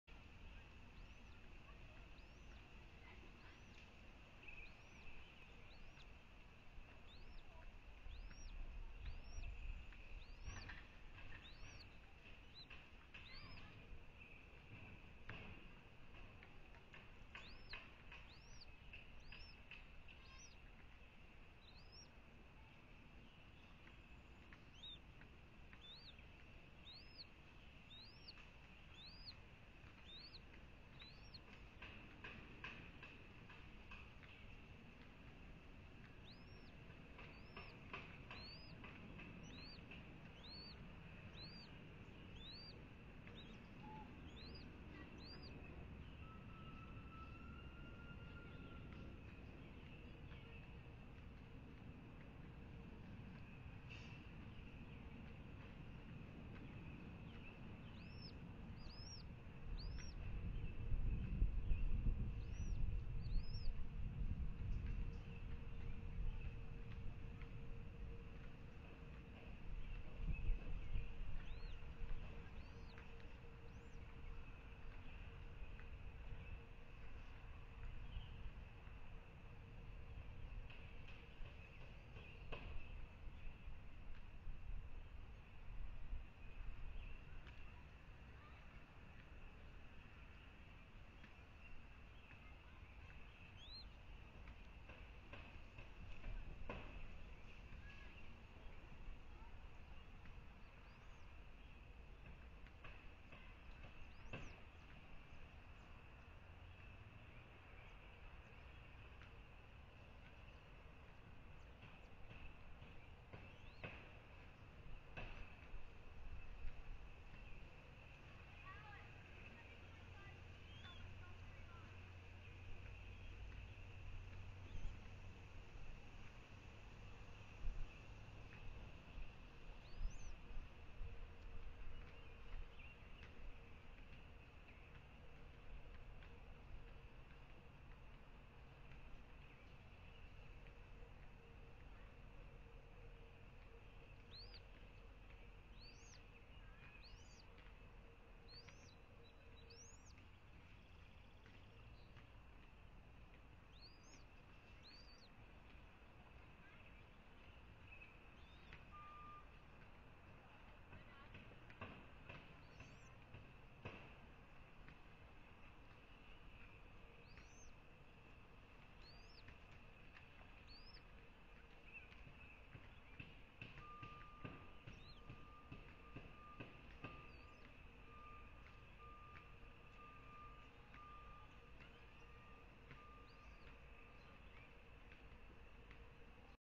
4-11-18 Presidio RTH Nest- 1st Feeding Of The Newly Hatched Chick
It was a very good feeding for a few hour old chick. Mom did a great job getting bites in, even if some were way, too big.  What an adorable face.